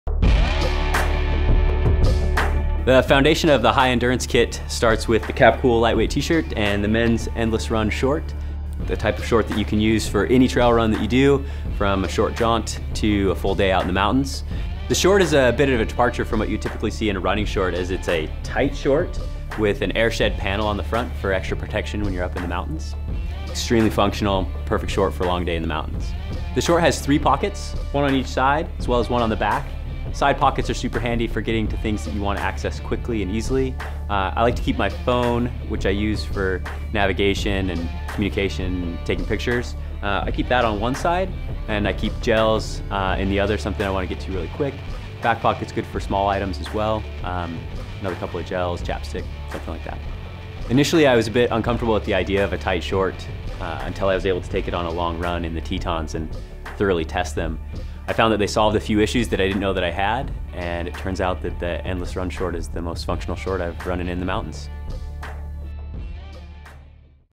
Patagonia® Men's Endless Run Shorts
These shorts were designed to minimize the need for additional pieces and streamline your kit. Two pockets built into the sides and one in the back let you redistribute the number of things in your pack and reduce bounce, for longer missions in the mountains. Fair Trade Certified™ sewn.